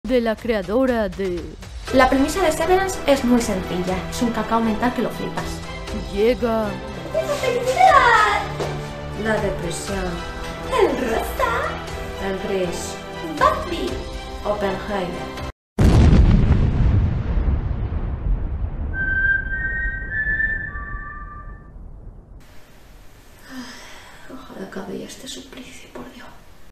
Barbenheimer promo
Si Barbie y Oppenheimer han sobrevivido a su estreno simultáneo, he pensado que a lo mejor mis críticas también pueden. HOY A PARTIR DE LAS 20:00 HORA ESPAÑOLA.

Y recordad, allscreeners...: Blue Beetle, 18 de agosto solo en cines ;D